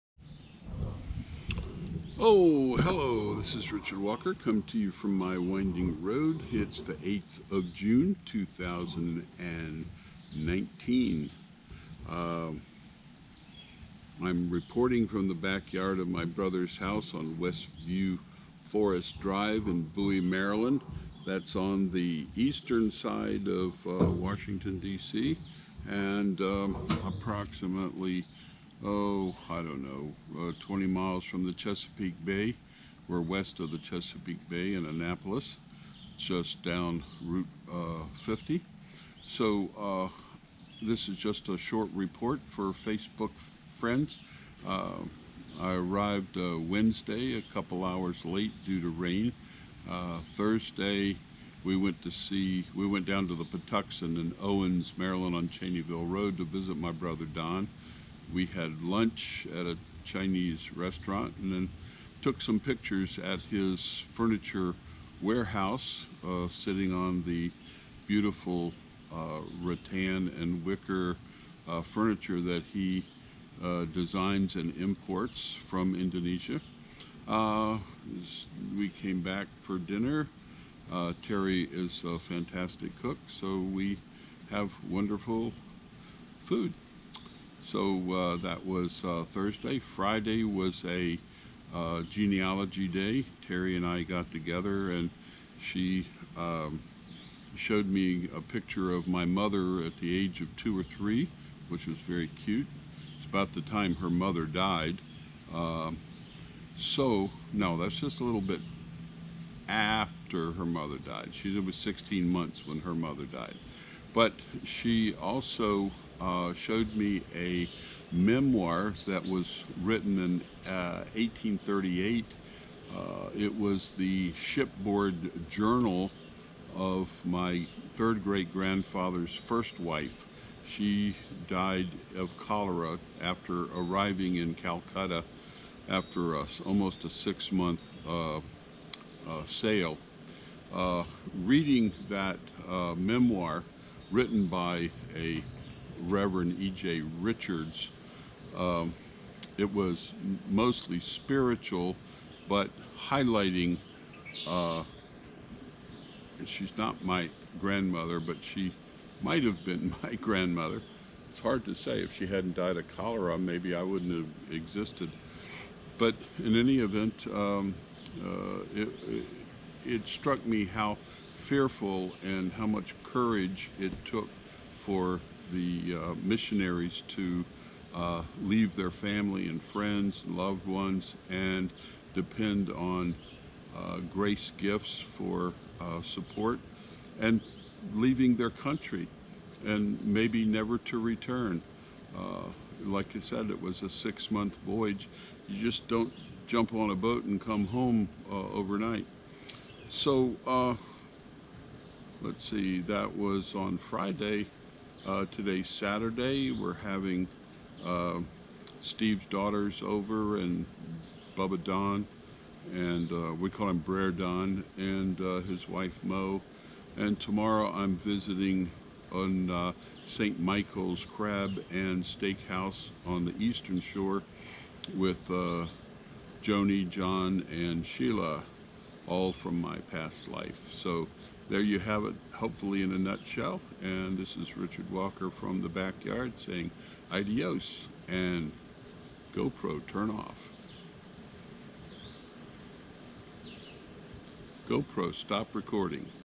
GH010147 Maryland trip short
Jun 3, 2019 Richard Walker flew to DCA National Airport to visit his two brother living in Bowie and Owings Maryland. In the video he itemizes his activities and events.  RAW